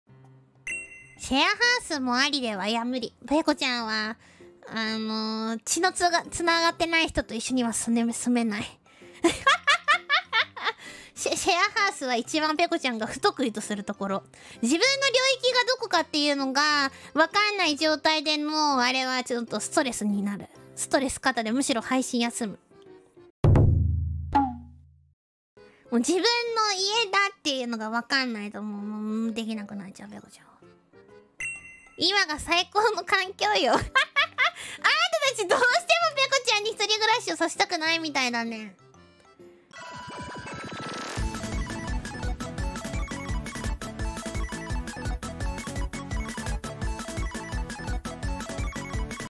Pekora Tells the Reason why She Doesn't Want to Live at a Sharehouse with other Holomembers
◆Original Stream:
【案件じゃない】「C」を消すだけで医療に貢献しちゃおう！！！！！ぺこ！【ホロライブ/兎田ぺこら】